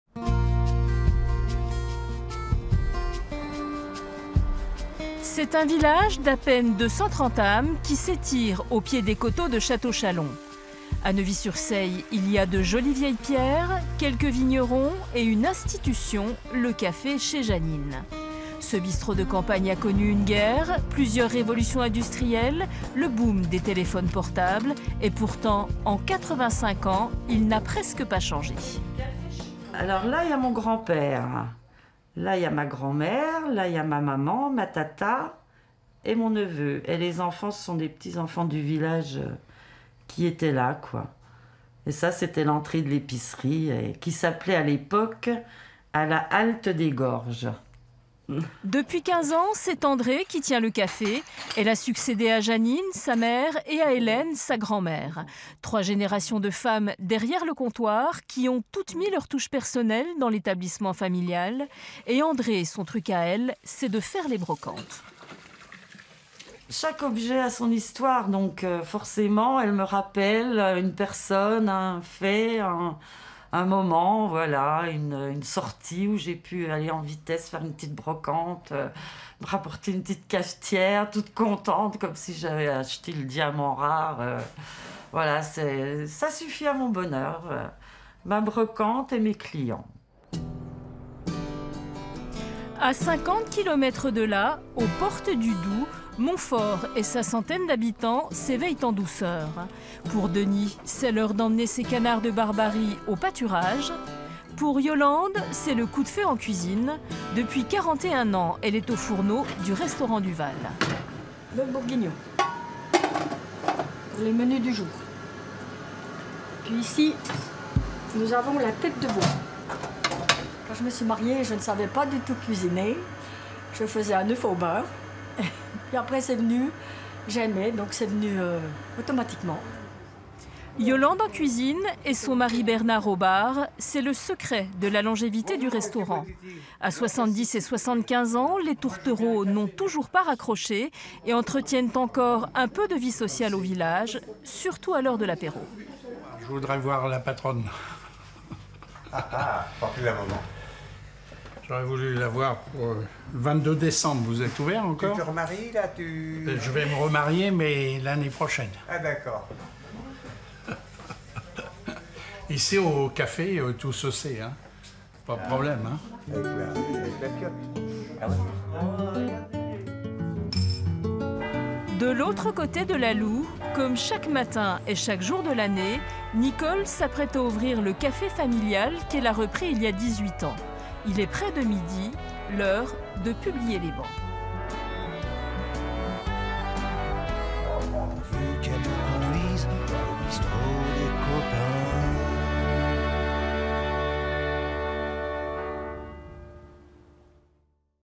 Episode 1 : À la découverte des derniers petits commerces ruraux de Franche-Comté
Un reportage de : PETIT Florence, BRACONNIER Elisabeth, MAYAYO Pierre, DUBUIS Emmanuel, BASCHUNG Marie et BARTOLI Quentin